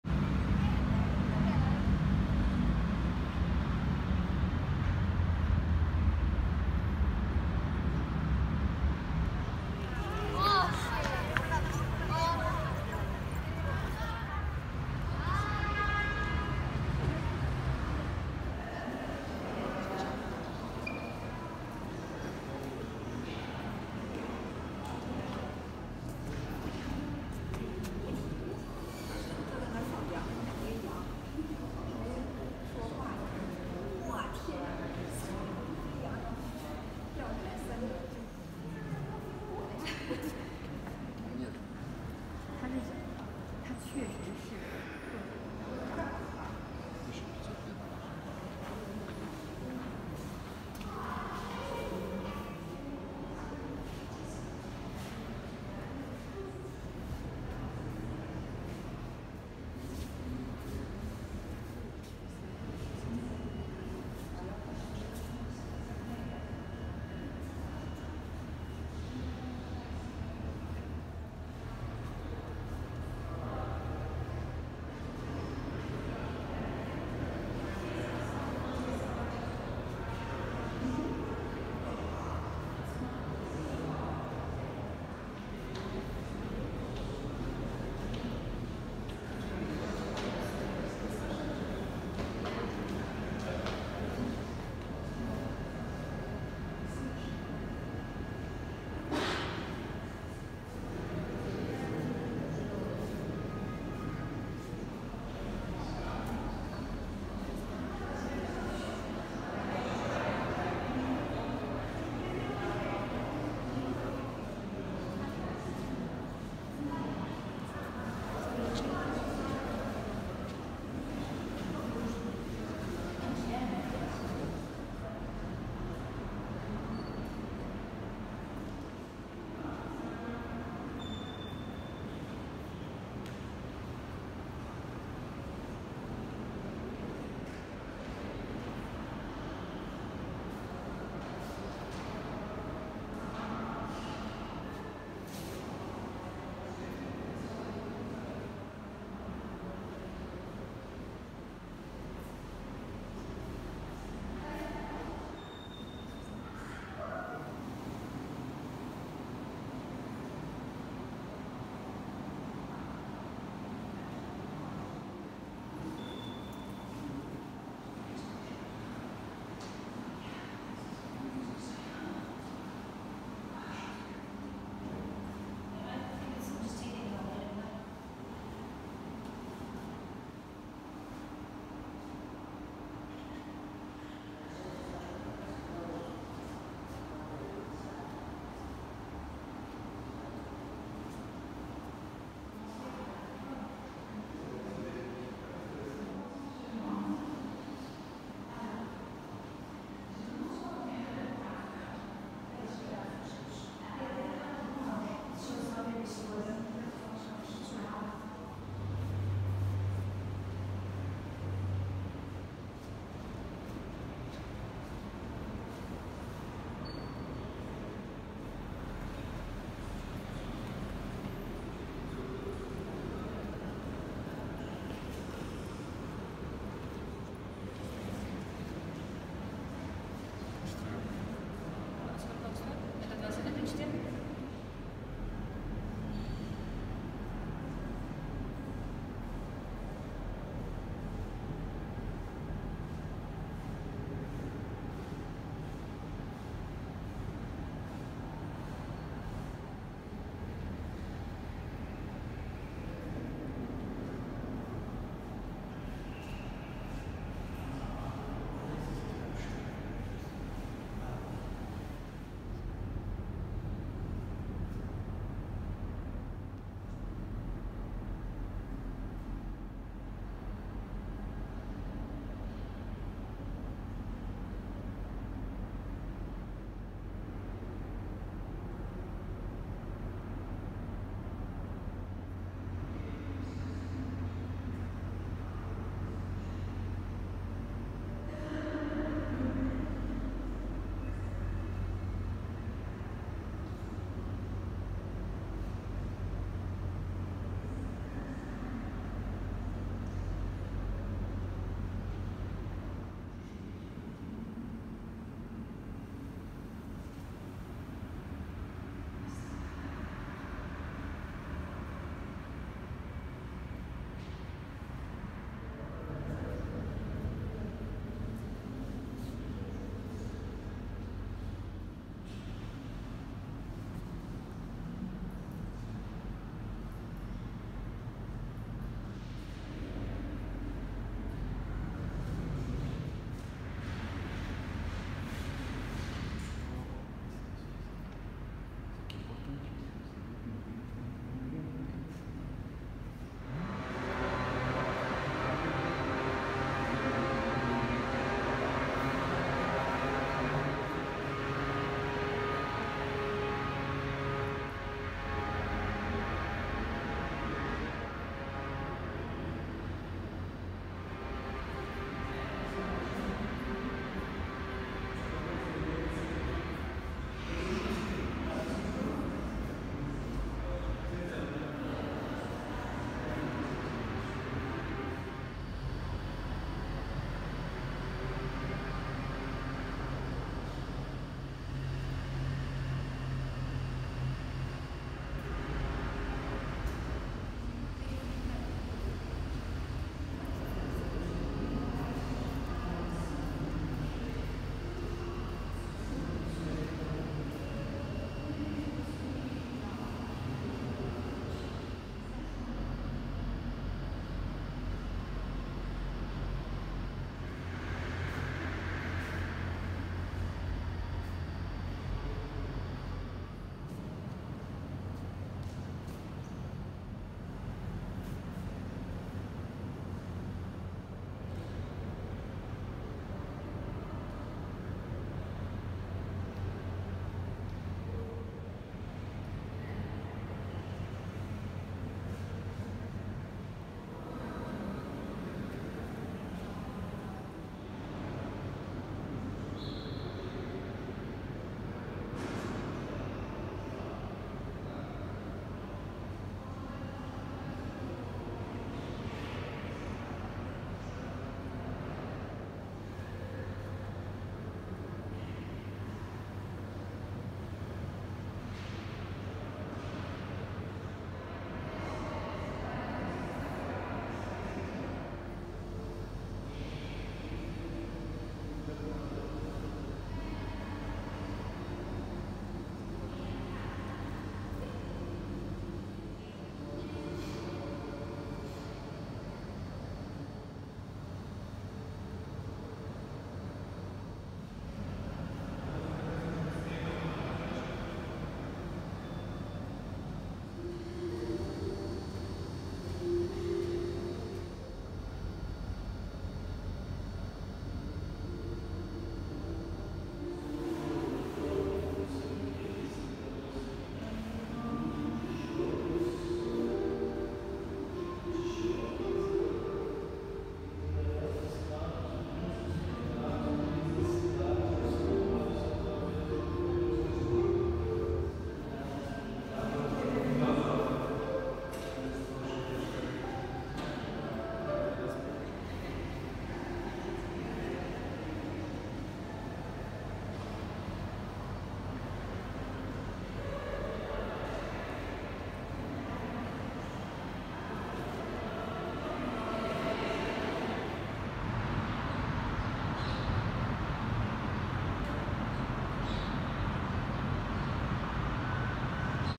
National Archaeological Museum of Athens - Highlights of the Exhibition
Highlights of the exhibition in the National Archaeological Museum of Athens, Greece. Some of the top pieces which can be seen in the video are the mask of Agamemnon, the bronze statue of Poseidon of Artemision, the jockey of Artemision.